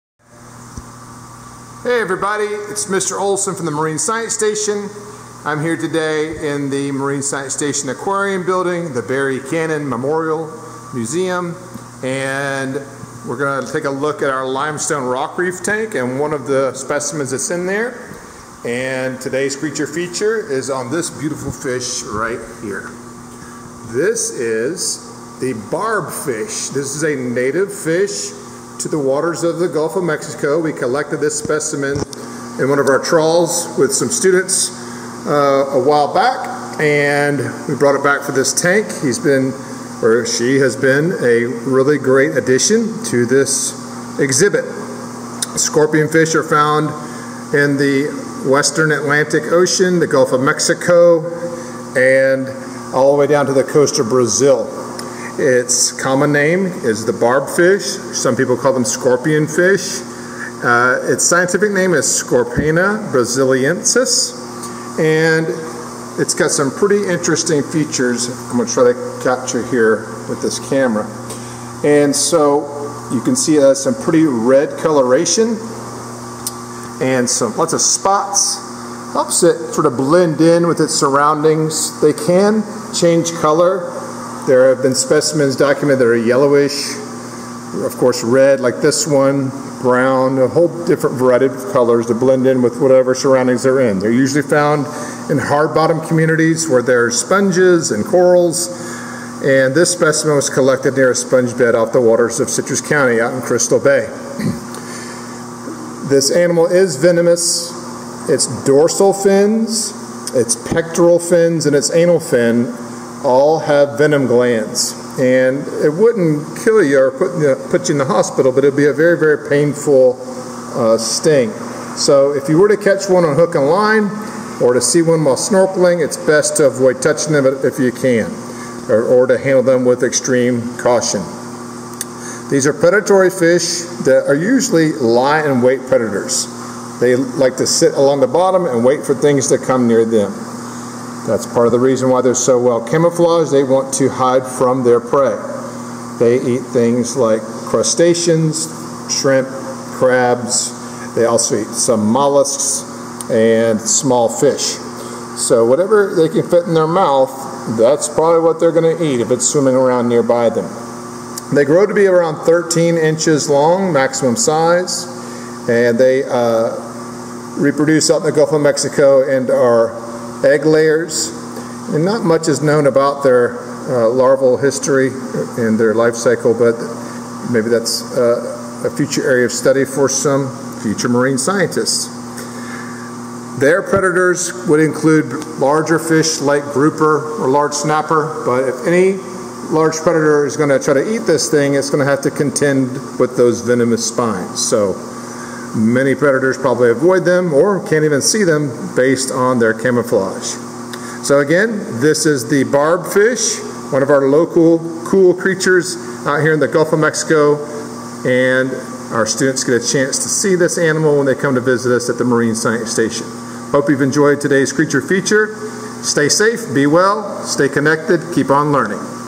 Creature Feature - Barbfish, Scorpaena brasiliensis, a venomous fish native to the Gulf of Mexico.
The Barbfish, also known as the Scorpionfish, is described in this video.  *Narration correction: the venom from this fish is injected via the dorsal, pelvic, and anal fins.